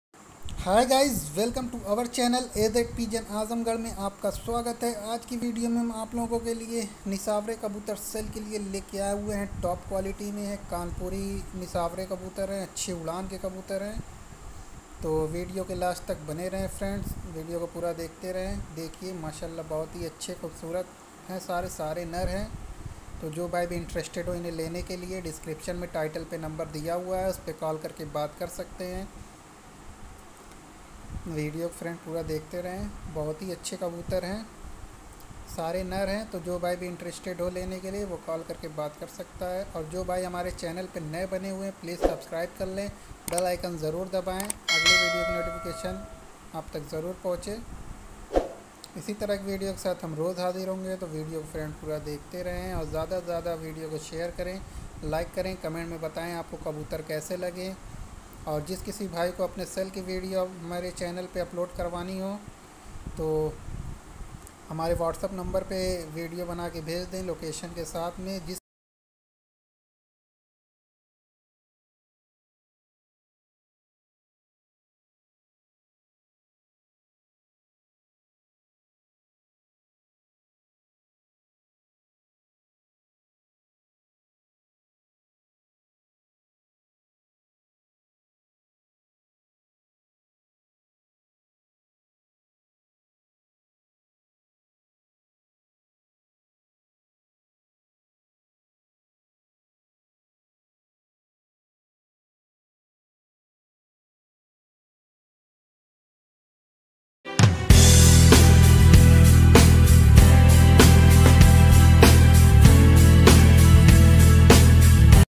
NISHWARA KABOOTAR FOR SALE - 7007634182#highflyerkabootar
Hey guys

AAJ KI VIDEO MA HUM AAP KA LIYA NISHWARA KABOOTAR SALE KA LIYA LAKAA AYAA HAI JO BHAI INTERESTED HO WO CALL KAR KA BAAT KAR SAKTA HAI

AZ PIGEON SHOP AZAMGARH

AZ PETS BIRDS SHOP AZAMGARH

THANKYOU